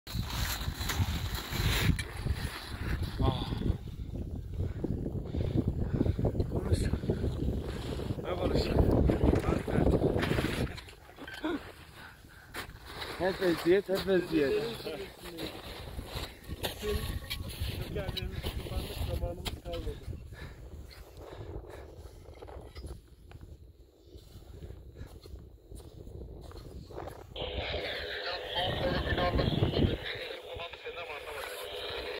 Niğde - Aladağlar - K. Çebel Zirve Yakınlarındayız
Zirve için planlanan zamanın biraz gerisinde kaldıgımız için zirve için son 100 metrelik yolu yükselmedik çünkü geç kalacaktık. Böyle bir aktivitenin içerisinde geri dönüş yolunda problem yaşamak istemiyorsanız kesinlikle planın dışına çıkmamalısınız!